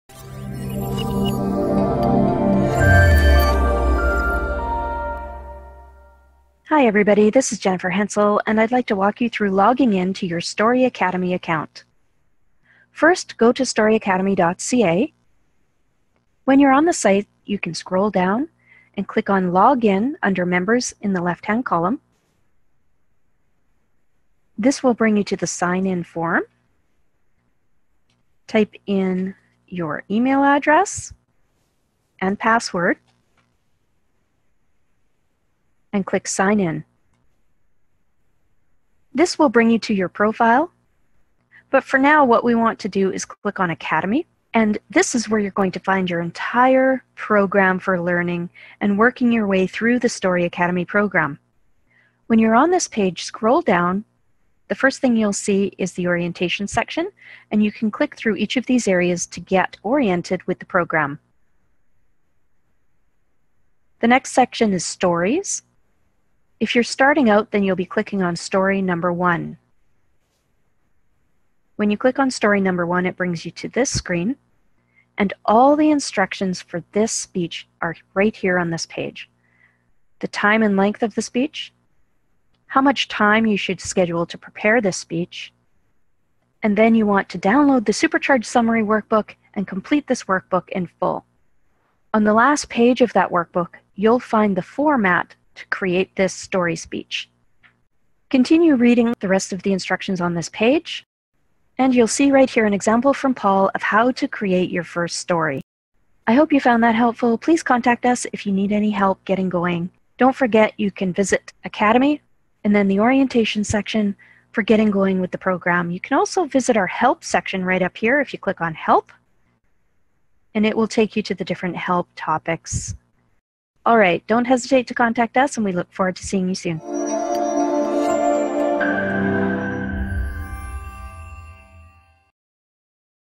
Story Academy: Login to Your Account
Member instructions for logging into your account and accessing Story #1 for the first time. If you are already a member, your account is automatically generated when you sign up. If you haven't signed up yet, visit the "Membership" section of our website to join Story Academy today! See you there.